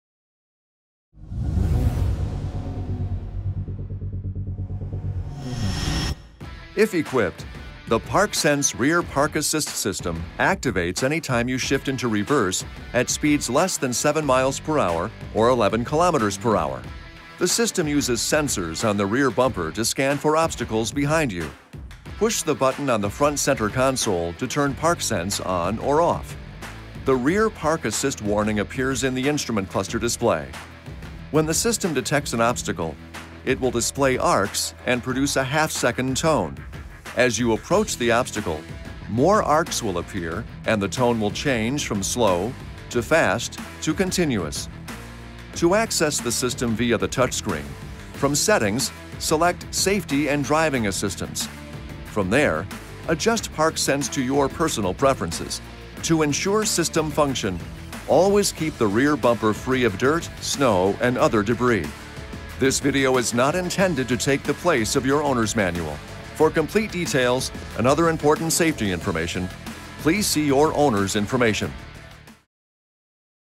ParkSense-How rear bumper sensors improve reverse parking safety in 2018 Dodge Charger
Learn how the sensor-based ParkSense Rear Park Assist feature identifies obstacles behind your vehicle for increased safety when parking your 2018 Dodge Charger. This feature will help you with reverse parking and parallel parking.